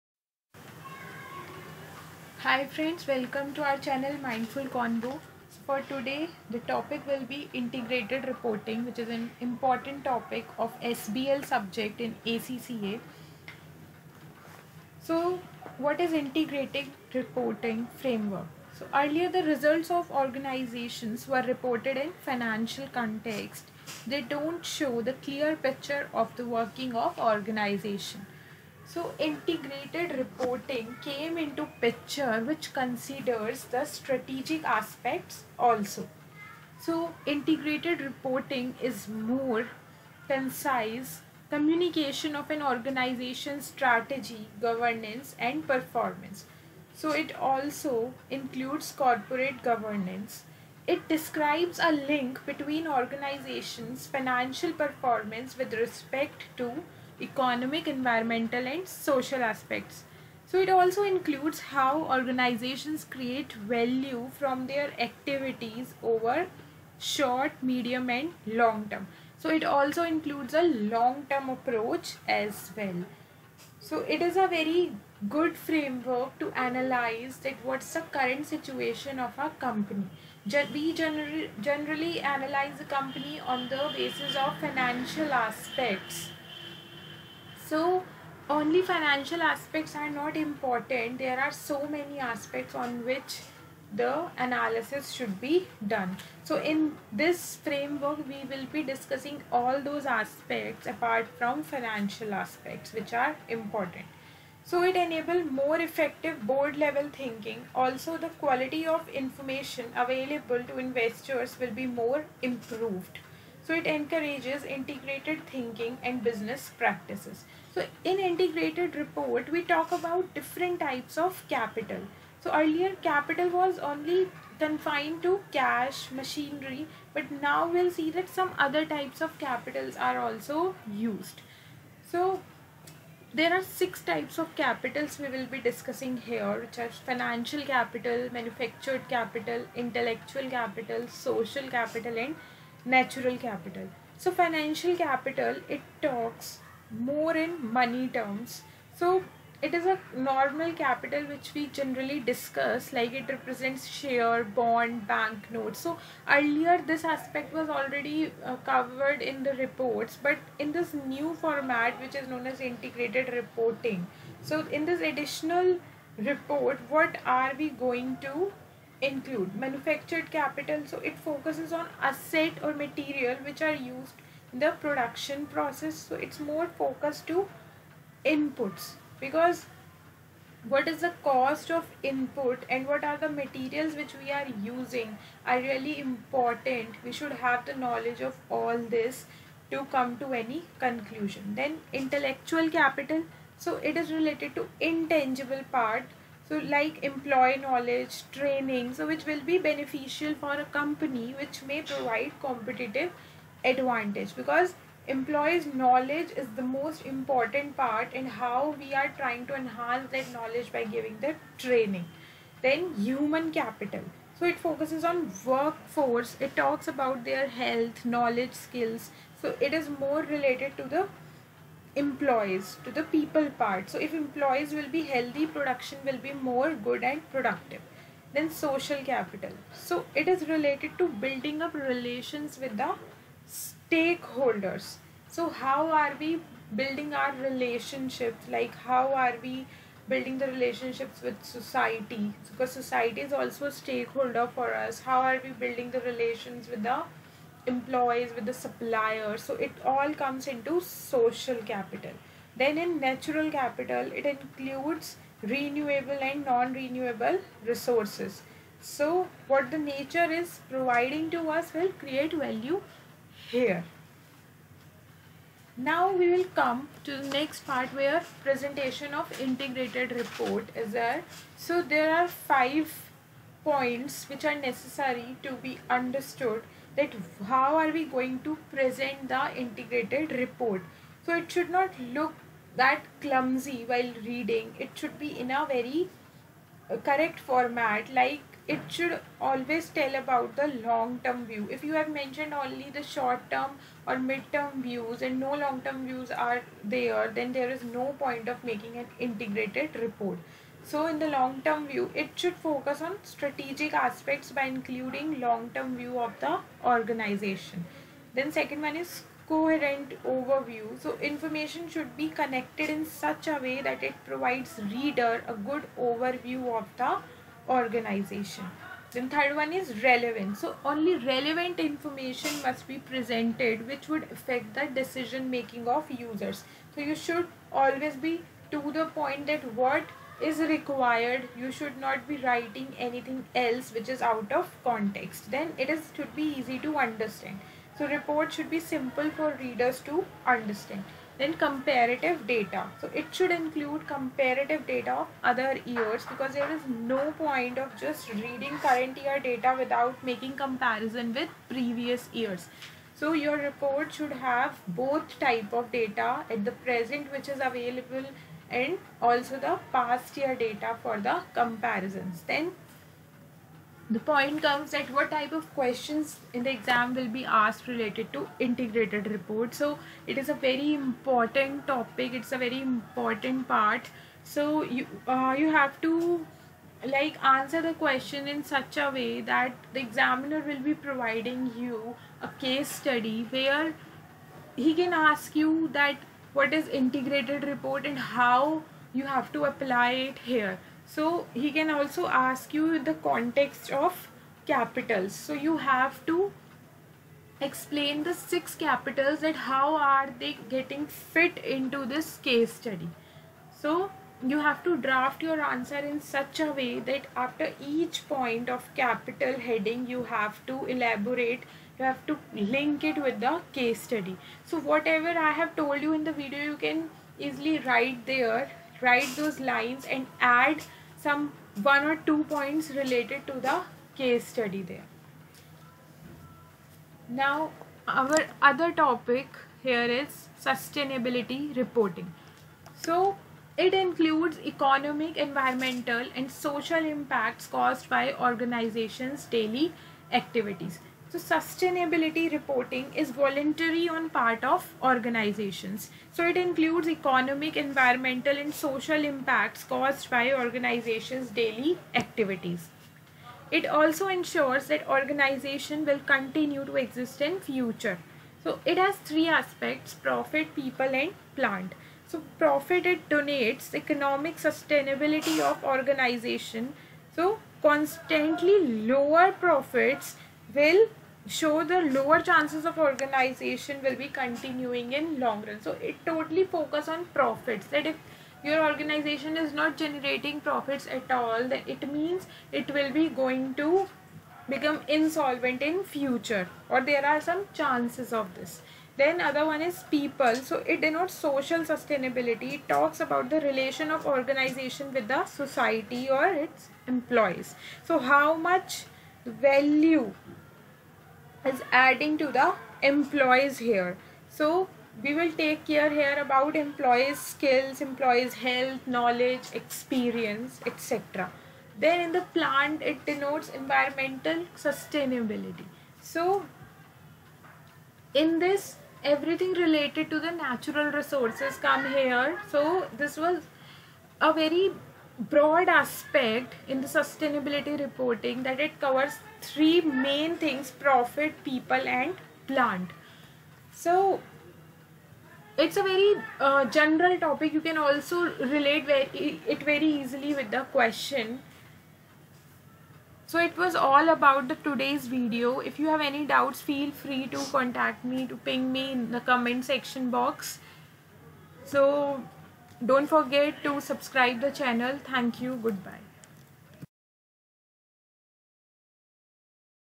INTEGRATED REPORTING| SUSTAINABILITY RREPORTING|ACCA SBL EXAM LECTURES| STRATEGIC BUSINESS LEADER
This video describes Integrated Reporting framework which is part of ACCA Strategic Business Leader Exam. This video also talks about capitals in Integrated reporting and how the report should be presented. Further this video also talks about sustainability reporting.